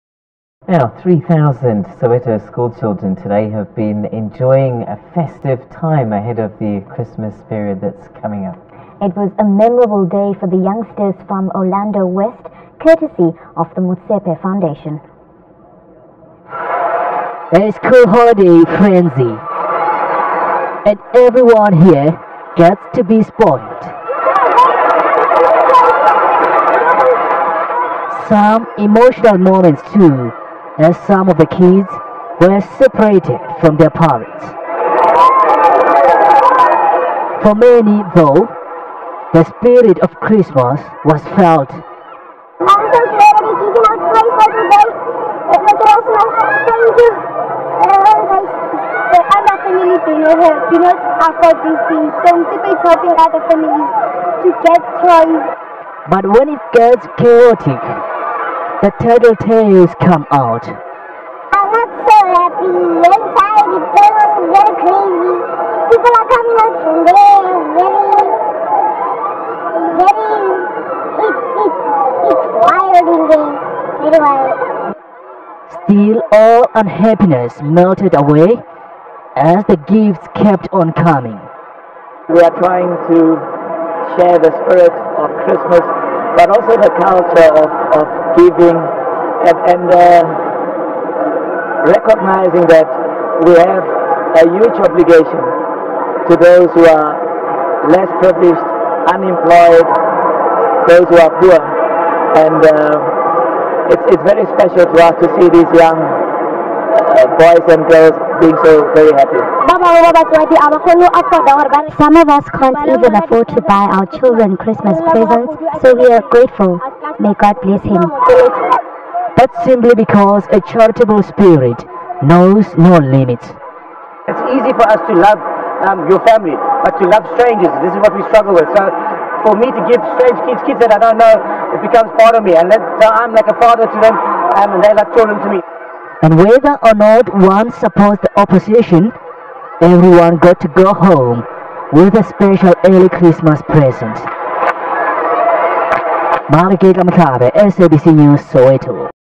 ☼ Christmas Has Come Early Soweto Children This Year Ta Updated
Christmas has come early for about 3000 Soweto children this year.
A memorable day for the youngsters from Orlando West,
whose Santa, came in the form of the Motsepe Foundation.